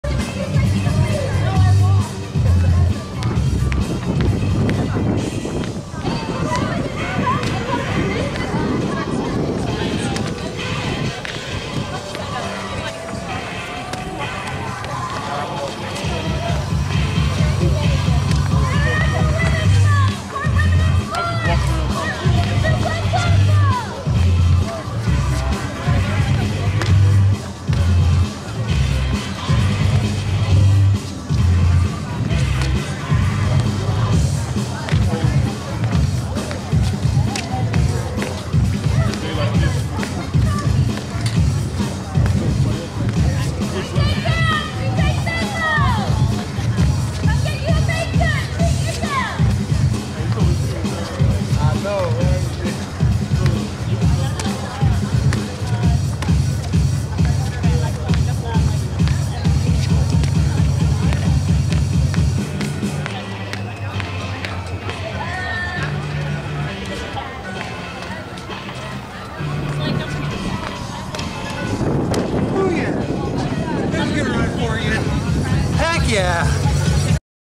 Tuesday Good Run
A sweet freestyle run during my typical Tuesday Hacky Sack session on Speakers Circle at Mizzou!